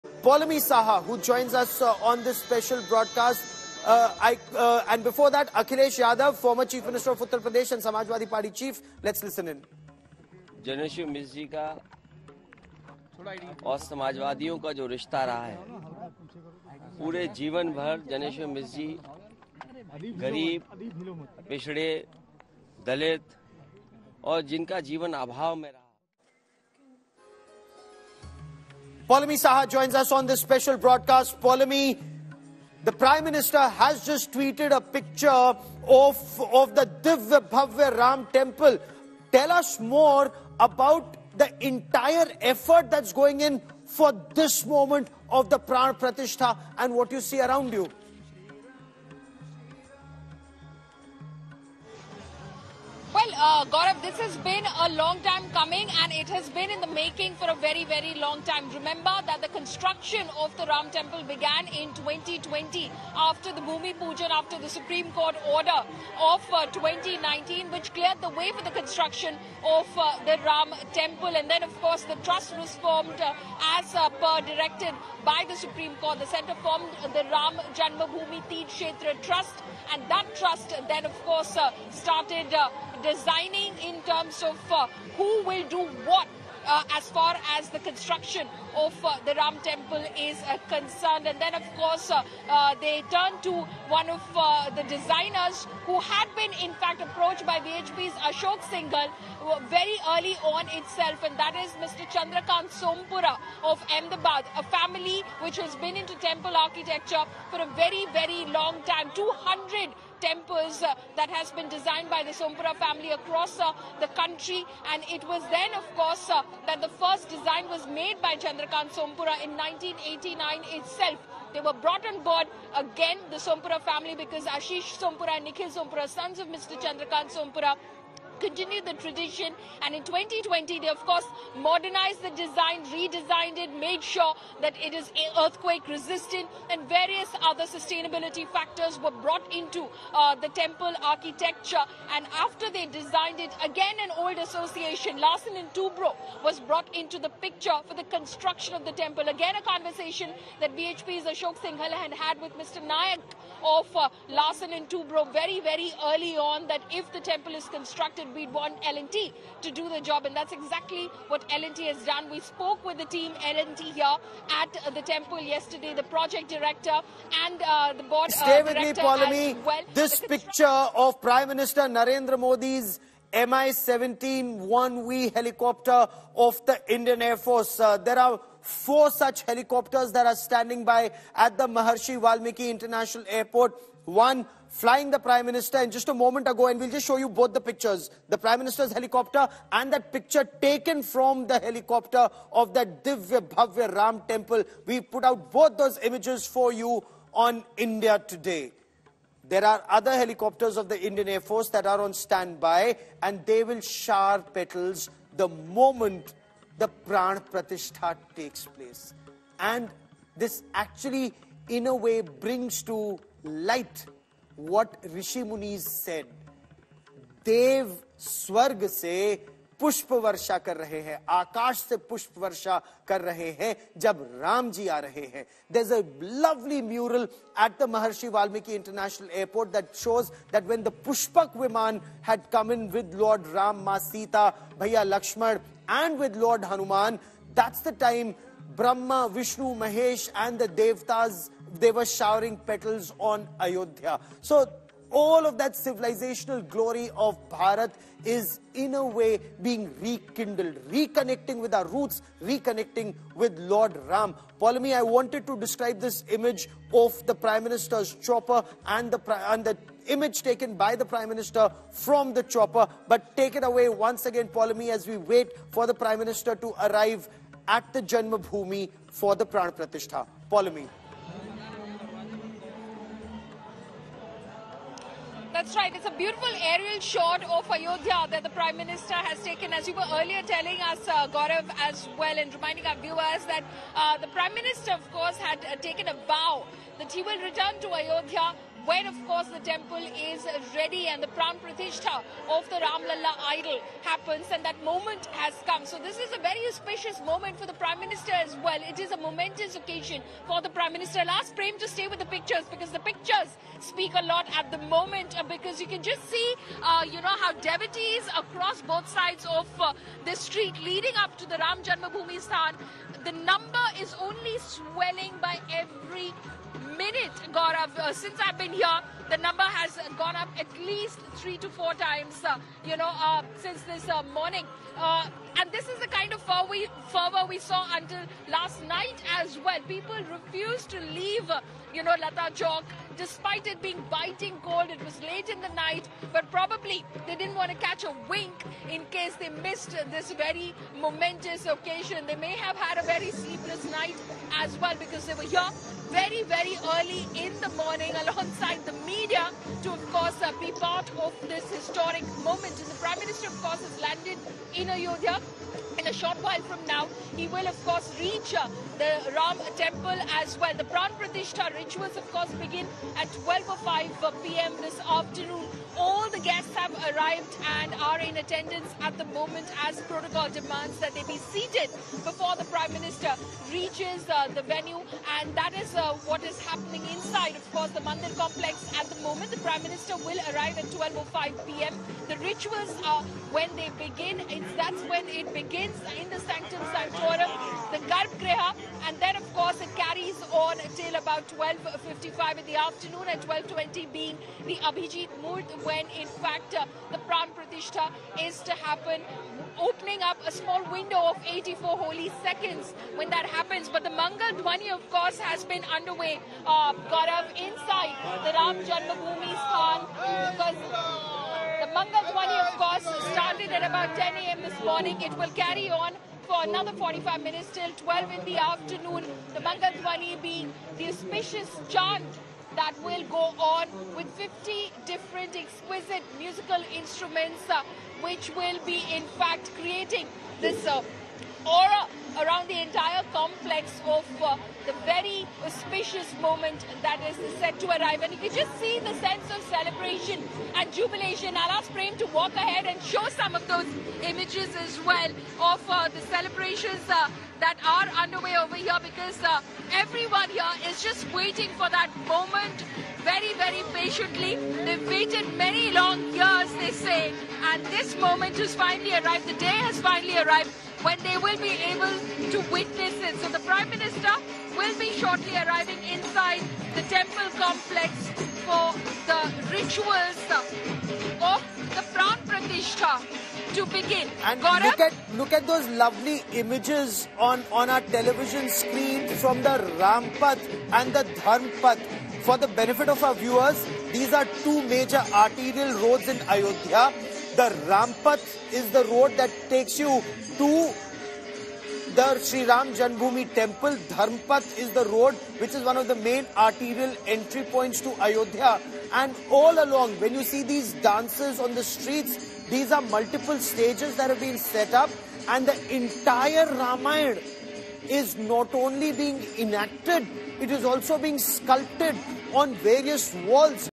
Watch: Bird's Eye View Of Ram Mandir From PM Modi's Plane Arriving In Ayodhya | Ram Mandir News
Ayodhya Ram Mandir Inauguration Updates: The inauguration of the Ram temple is a turning point in India's cultural history, culminating a journey spanning centuries that witnessed several legal and political events. As the pran pratishtha of the idol of Lord Ram takes place, follow for all the latest updates as we bring you exclusive coverage of the grand event from Ayodhya.